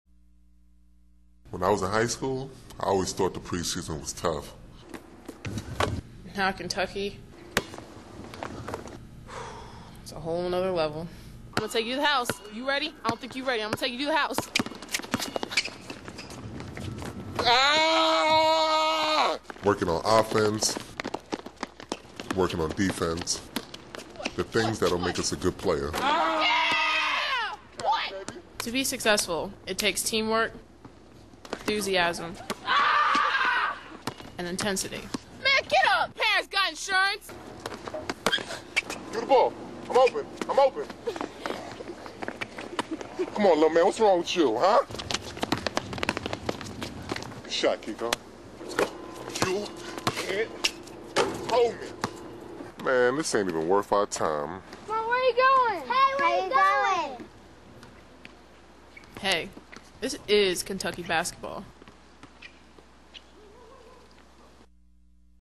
This is Kentucky Basketball - On the Court
"Commercial" shown at the 2004 Big Blue Madness designed after the "This is Sportscenter" commercials